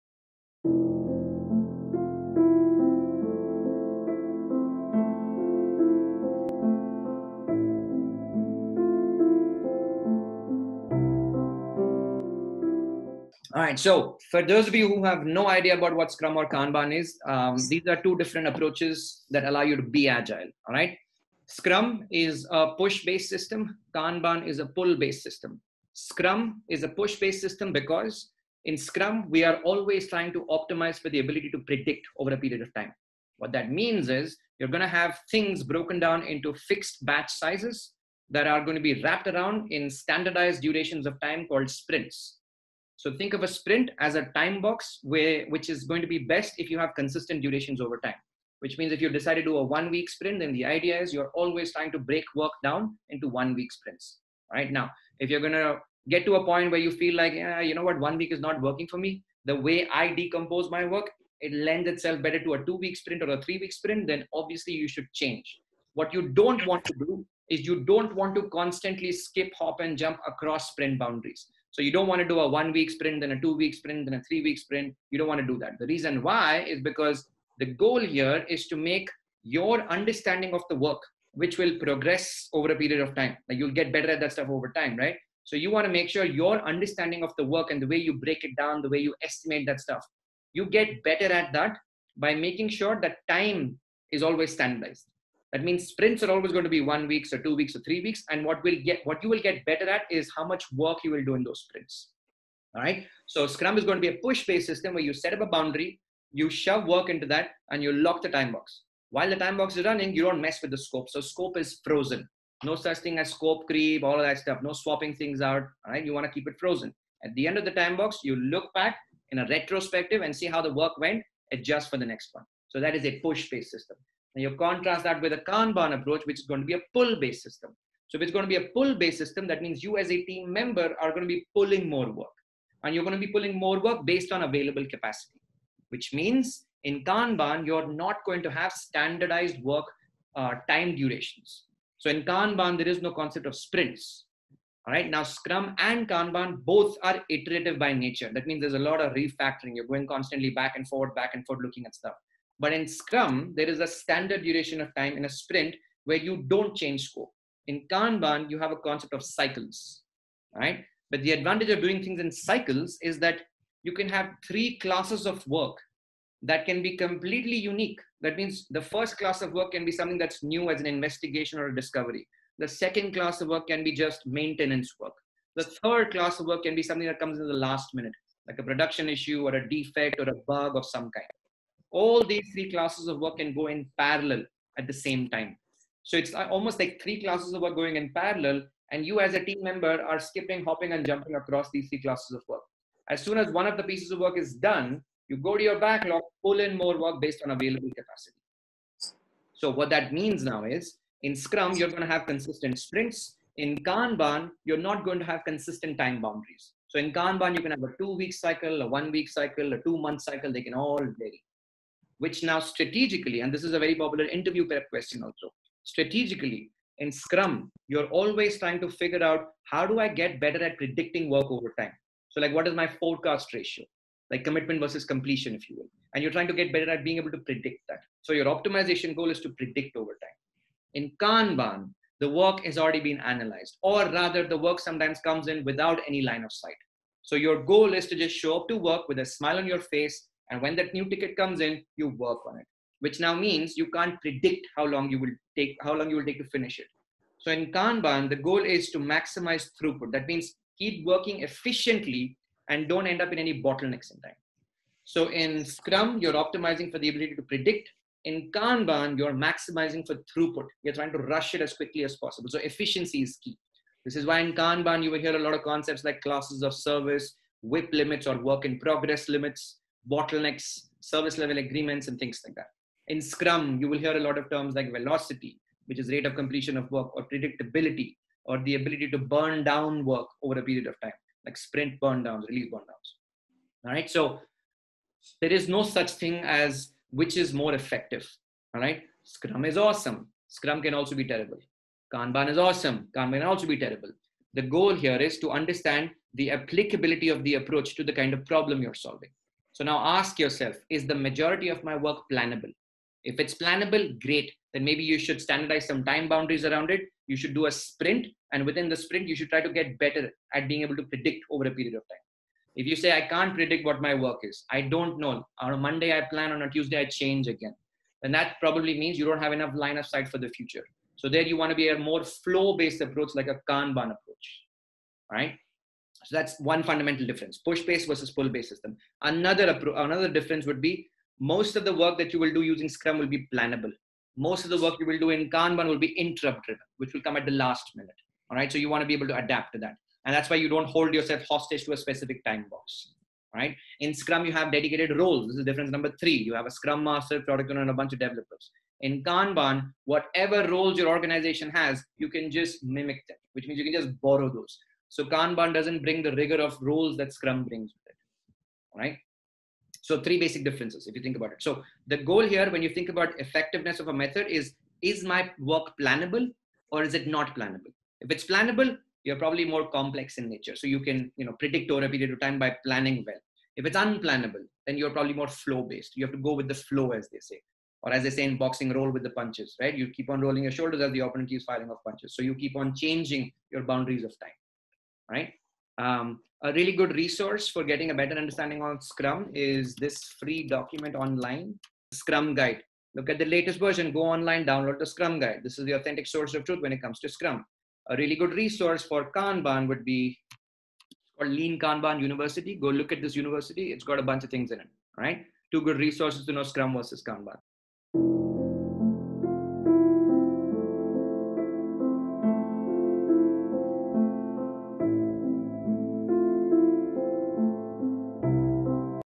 Scrum Vs Kanban
This SimpliAxis Webinar Question and Answer Section Video Mr. Aakash talking about difference between Scrum and Kanban